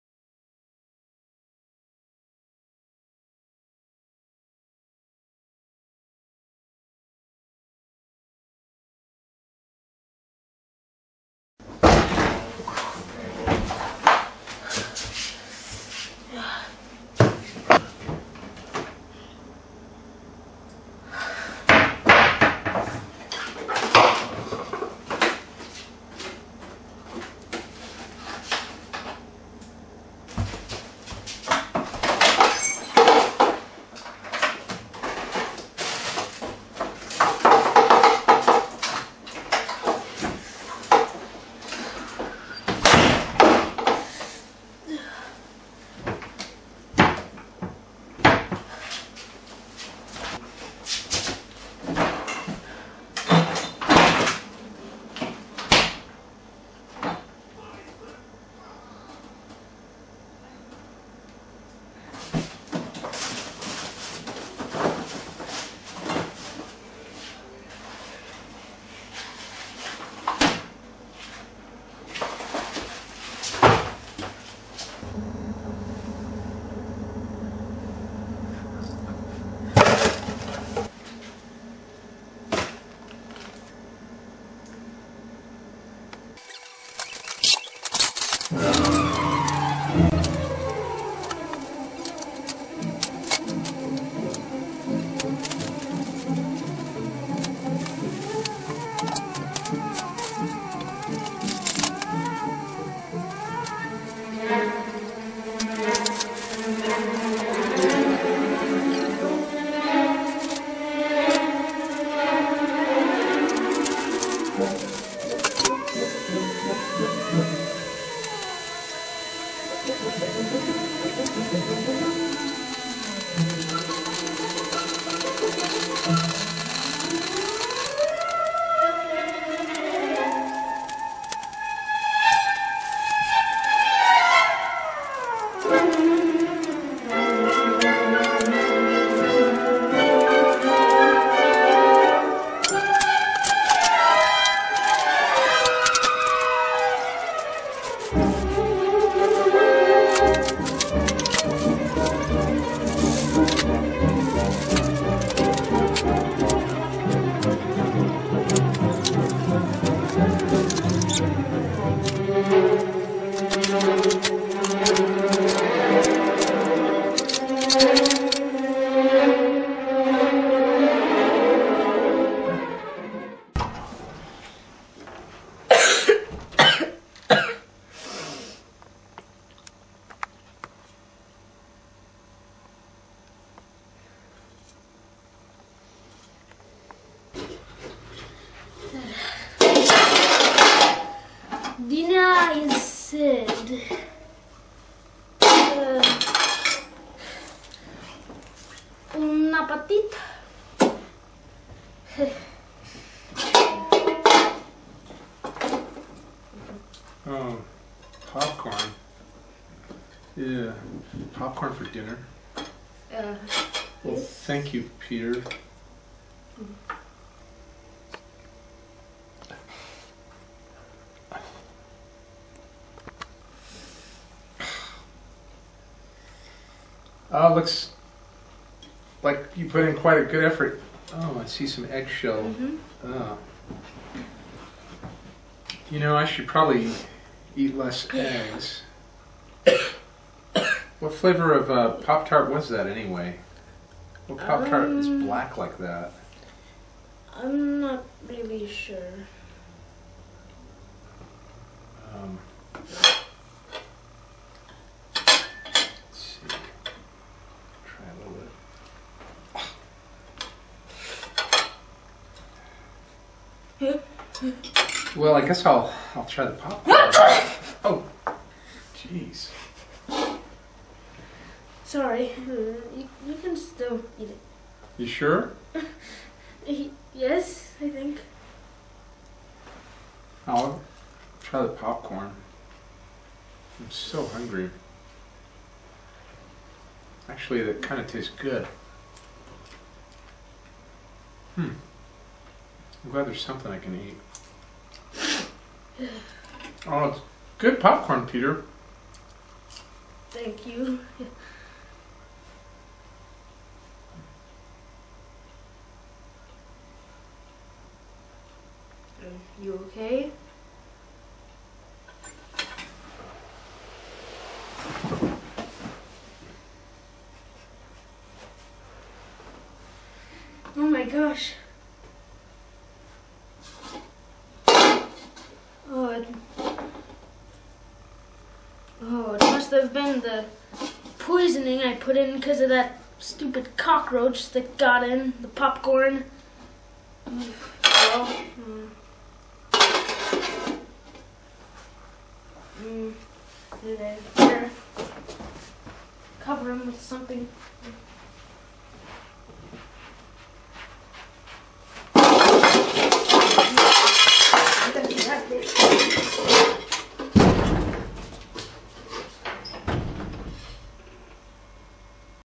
Wozope TV- Chef Peter
A boy only has an hour to make dinner before his dad gets home, and he isn't the best cook.